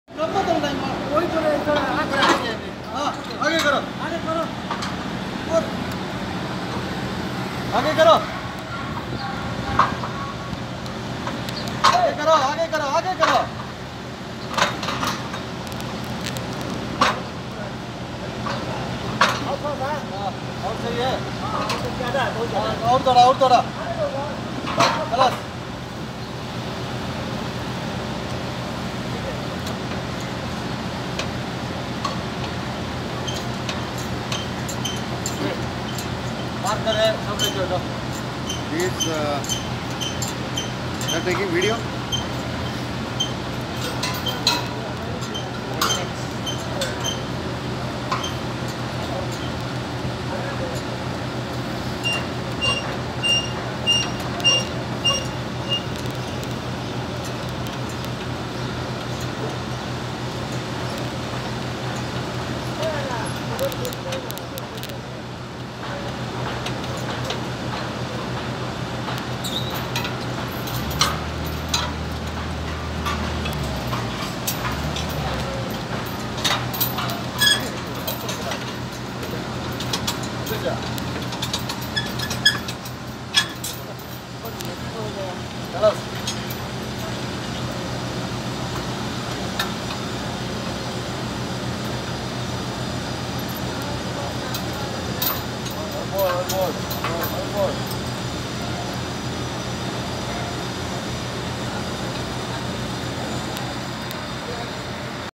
CABLE DRUM with STAND JACK LIFTERS CABLE PULLING.
all my intent has no any harm to the public pure entertainment and learning. Jack lifters cable drum during cable pulling.